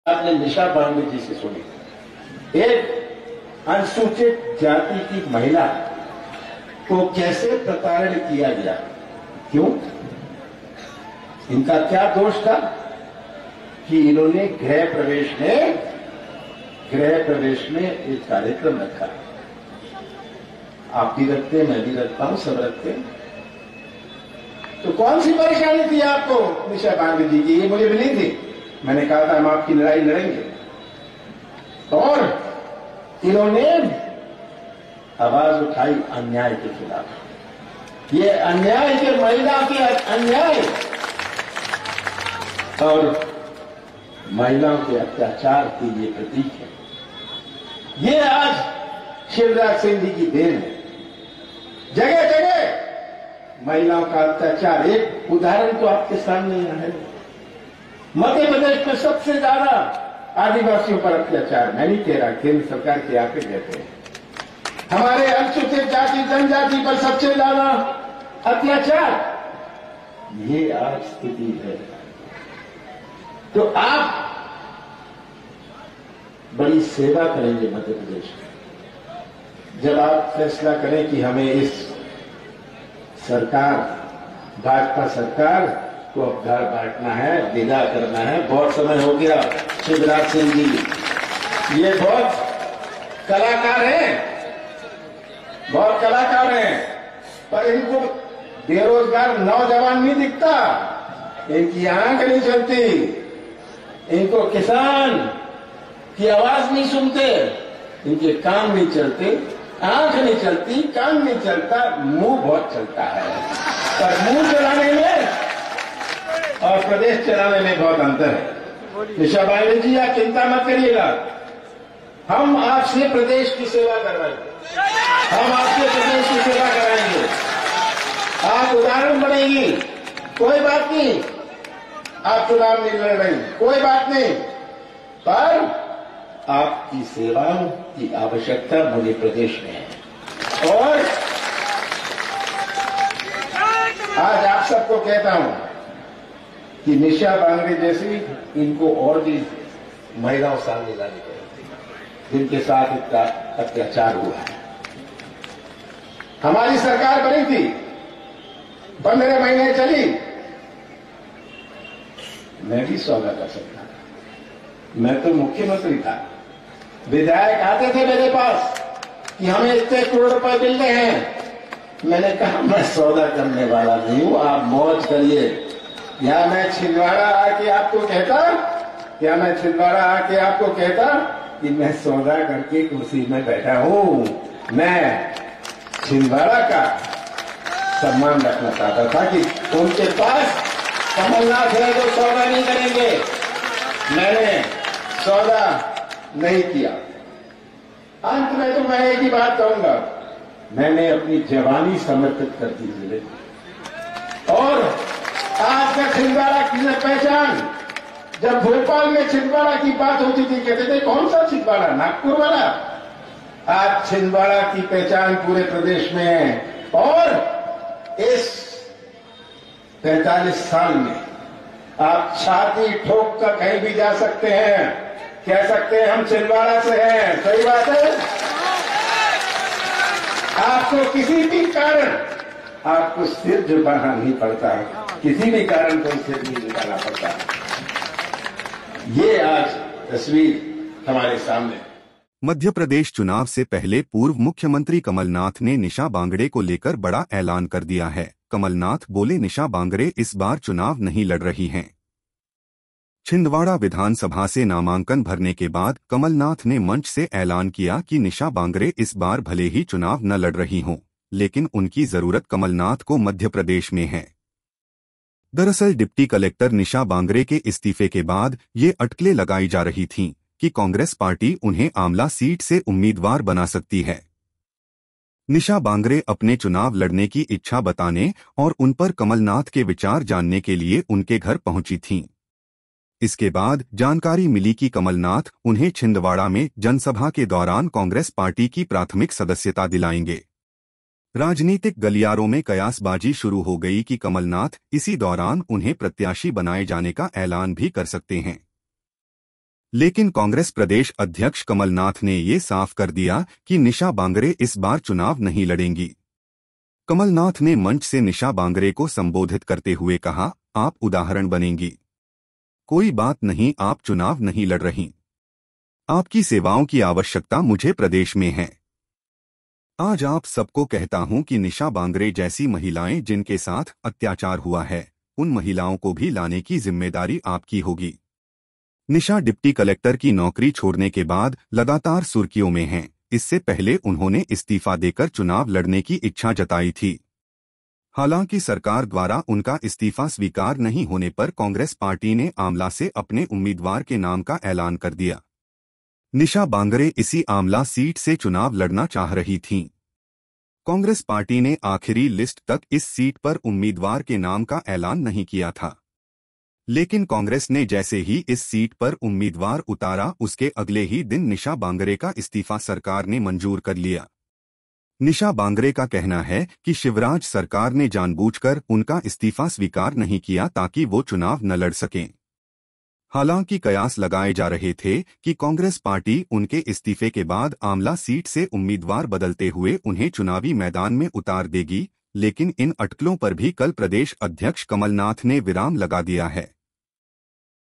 डिप्टी कलेक्टर निशा बांगरे से क्या बोले पूर्व CM कमलनाथ, छिन्दवाड़ा जनसभा से लाइव सुनिए | MP Election
पूर्व SDM निशा बांगरे के कांग्रेस में शामिल होने के बाद, पूर्व CM कमलनाथ छिन्दवाड़ा से कर रहे हैं, बड़ा ऐलान, देखिये पूरा विडियो

Former MP CM Kamal Nath spoke about the Congress ticket for the Ex-deputy collector Nisha Bangre at the Chhindwara rally in Madhya Pradesh.